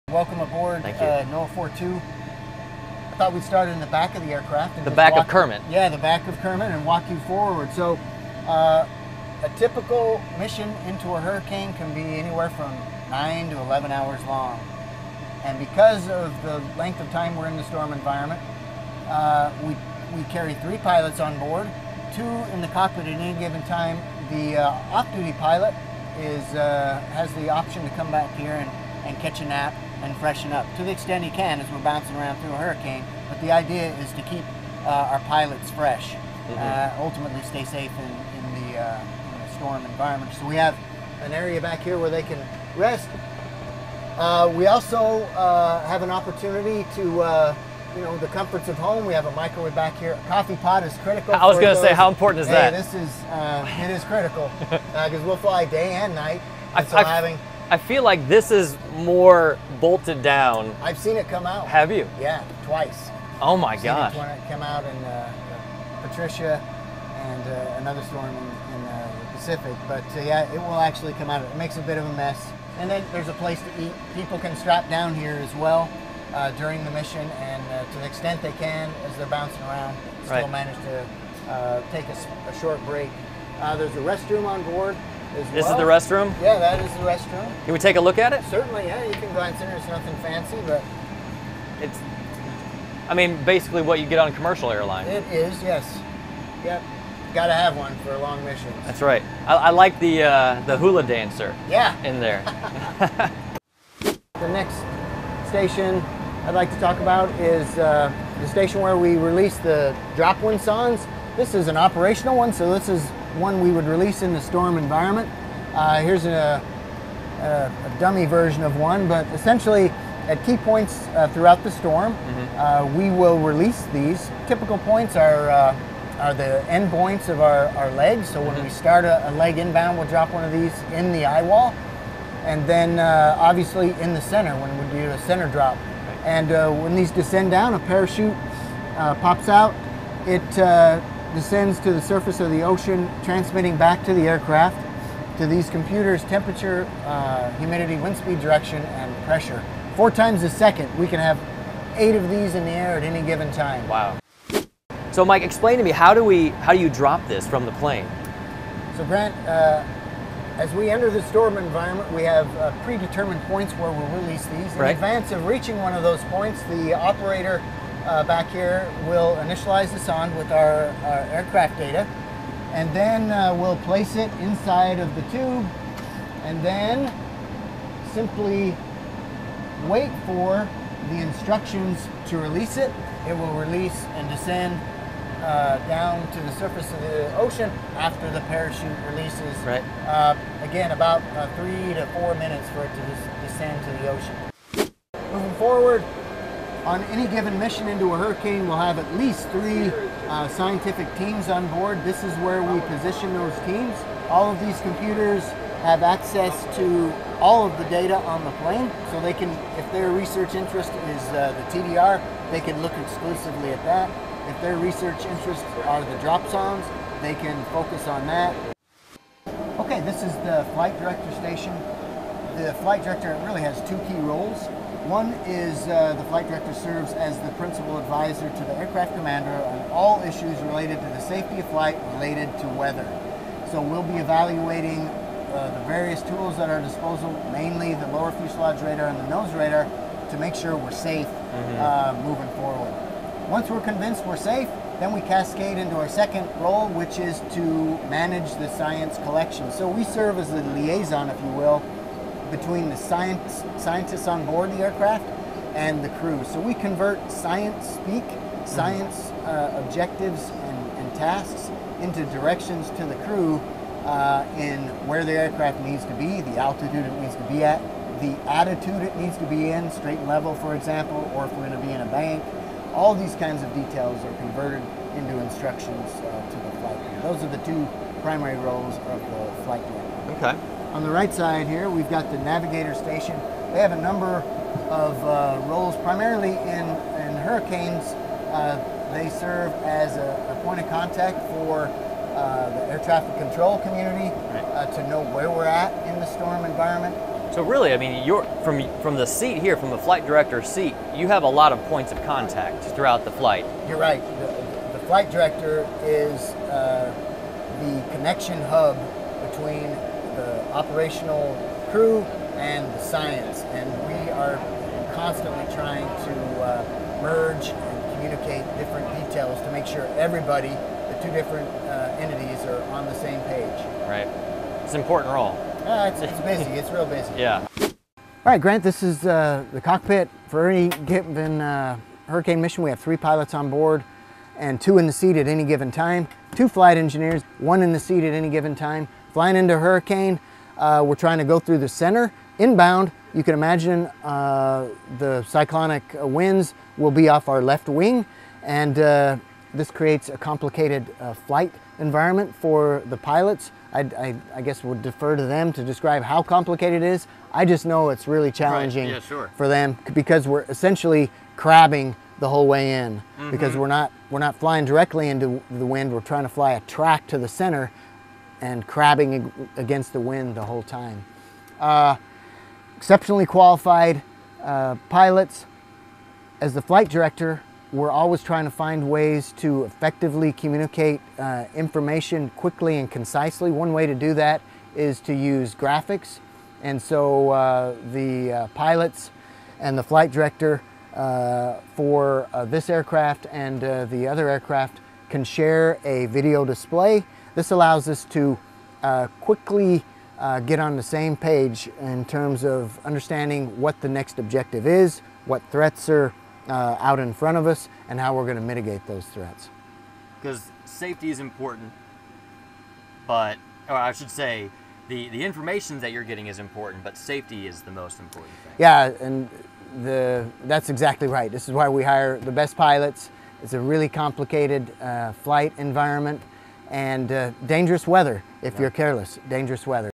Inside NOAA Hurricane Hunters' plane: It's a research lab in the middle of a monster | 10News WTSP
Inside NOAA's Lockheed WP-3D Orion aircraft, a team works together to safely navigate through a hurricane and collect the essential information needed to compute better forecasts. Look for a few items from "home," too, including a microwave, coffee pot and a place to sleep.